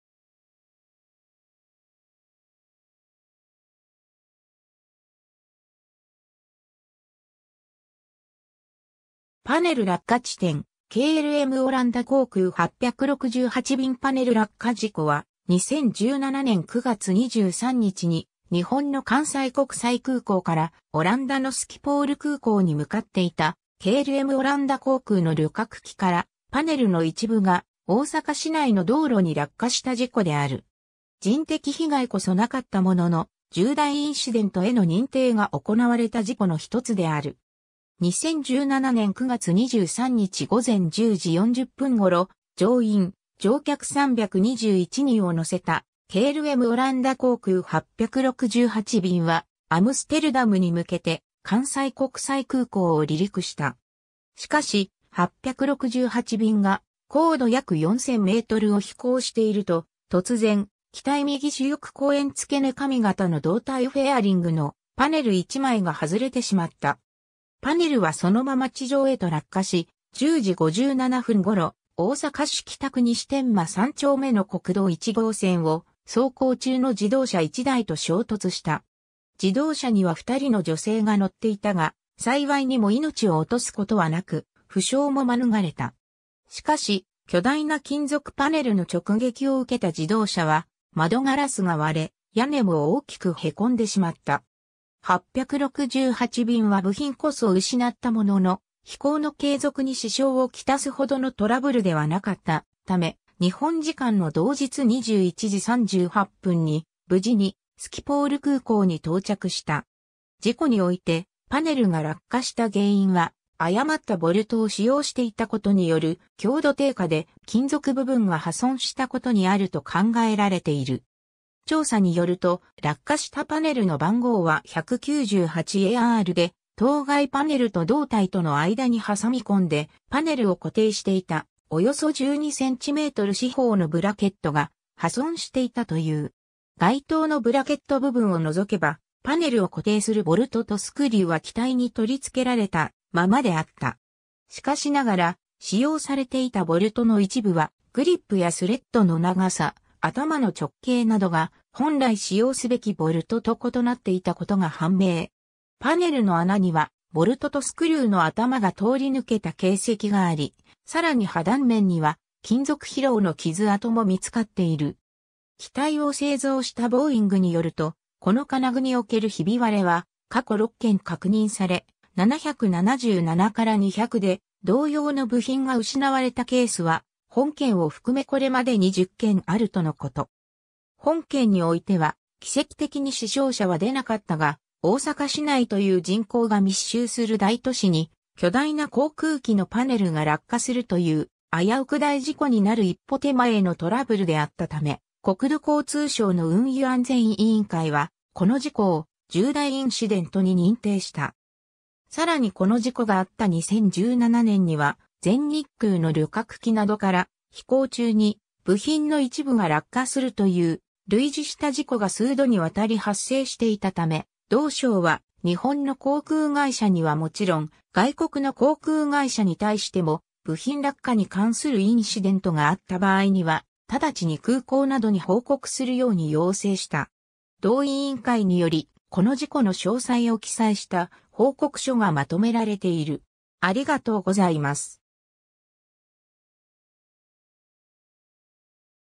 KLMオランダ航空868便パネル落下事故
パネル落下地点 KLMオランダ航空868便パネル落下事故（KLMオランダこうくう868びんパネルらっかじこ）は、2017年9月23日に、日本の関西国際空港から、オランダのスキポール空港に向かっていたKLMオランダ航空の旅客機から、パネルの一部が大阪市内の道路に落下した事故である。
人的被害こそなかったものの「重大インシデント」への認定が行われた事故の1つである。
 2017年9月23日午前10時40分頃、乗員・乗客321人を乗せたKLMオランダ航空868便（機種：ボーイング777-200ER・レジ番：PH-BQC）は、アムステルダムに向けて、関西国際空港を離陸した。
しかし、868便が高度約4,000 mを飛行していると、突然、機体右主翼後縁付け根上方の胴体フェアリング（整流板）のパネル（重量約4.3 kg・縦横の幅共に1 m前後・厚さ約4 mm）1枚がはずれてしまった。
 パネルはそのまま地上へと落下し、10時57分ごろ、大阪市北区西天満3丁目の国道1号線（北緯34度41分51秒・東経135度30分27秒）を走行中の自動車1台と衝突した。
自動車には2人の女性が乗っていたが、幸いにも命を落とすことはなく、負傷も免れた。
しかし、巨大な金属パネルの直撃を受けた自動車は、窓ガラスが割れ、屋根も大きく凹んでしまった。
 868便は部品こそ失ったものの、飛行の継続に支障をきたす程のトラブルではなかったため、日本時間の同日21時38分に、無事にスキポール空港に到着した。
 事故において、パネルが落下した原因は、誤ったボルトを使用していたことによる強度低下で、金属部分が破損したことにあると考えられている。
 調査によると、落下したパネルの番号は198ARで、当該パネルと胴体との間に挟み込んでパネルを固定していた、およそ12 cm四方のブラケット（取付け金具 P/N:149W5913-4）が破損していたという。
該当のブラケット部分を除けば、パネルを固定するボルトとスクリューは機体に取り付けられたままであった。
しかしながら、使用されていたボルトの一部は、グリップやスレッドの長さ、頭の直径などが、本来使用すべきボルトと異なっていたことが判明。
パネルの穴にはボルトとスクリューの頭が通り抜けた形跡があり、さらに破断面には金属疲労の傷跡も見つかっている。
 機体を製造したボーイングによると、この金具におけるひび割れは過去6件確認され、777-200（同型機）で同様の部品が失われたケースは、本件を含めこれまでに10件あるとのこと。
 本件においては奇跡的に死傷者は出なかったが、大阪市内という人口が密集する大都市に巨大な航空機のパネルが落下するという、危うく大事故になる一歩手前のトラブルであったため、国土交通省の運輸安全委員会は、この事故を「重大インシデント」に認定した。
 さらにこの事故があった2017年には、全日空の旅客機などから、飛行中に部品の一部が落下するという、類似した事故が数度にわたり発生していたため、同省は日本の航空会社にはもちろん、外国の航空会社に対しても、部品落下に関するインシデントがあった場合には、直ちに空港などに報告するように要請した。
 同委員会により、この事故の詳細を記載した報告書がまとめられている。